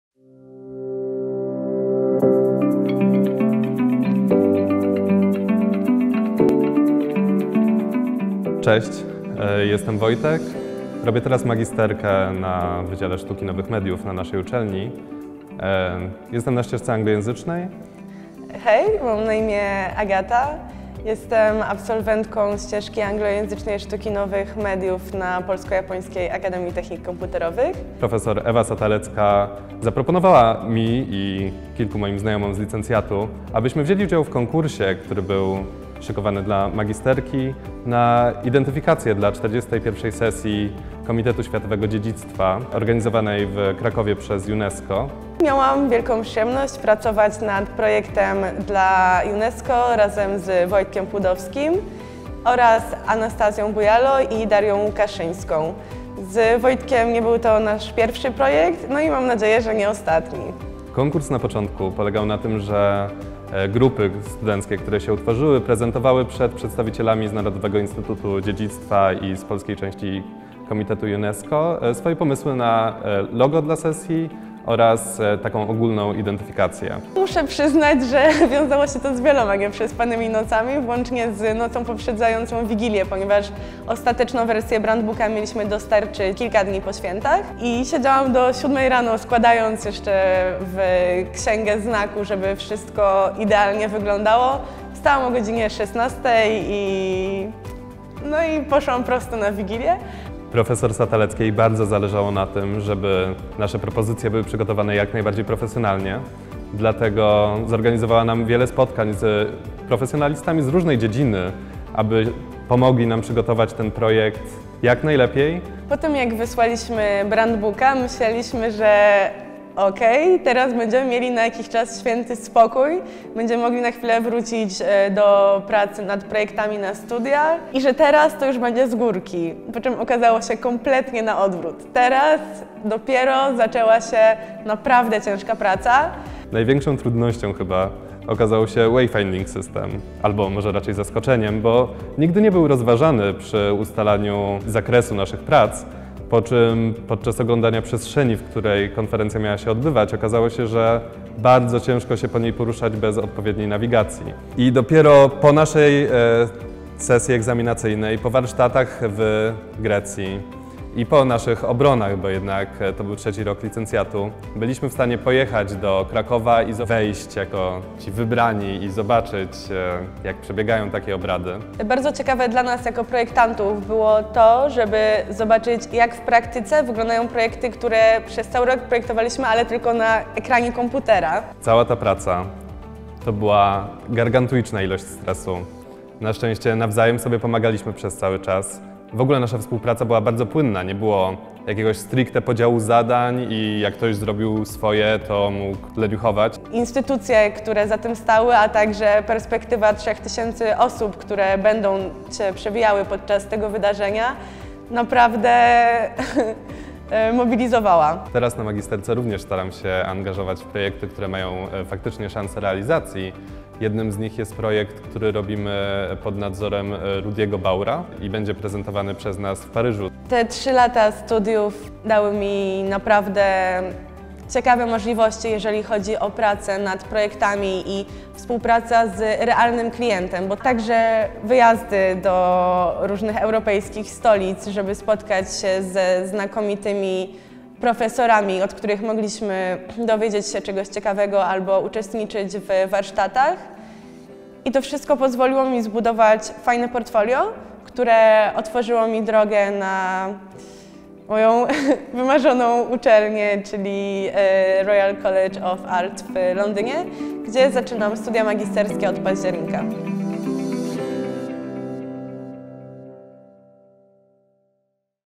Agata Juszkiewicz i Wojciech Płudowski - Project Spotlight - UNESCO
Nowością trzeciego odcinka jest dwójka bohaterów - Agata Juszkiewicz i Wojciech Płudowski. Stworzyli oni, w sumie w   czteroosobowym zespole,ogromny projekt -  identyfikację dla 41 sesji Komitetu Światowego Dziedzictwa UNESCO, która odbyła się w zeszłym roku w Krakowie. Photo by 41WHC UNESCO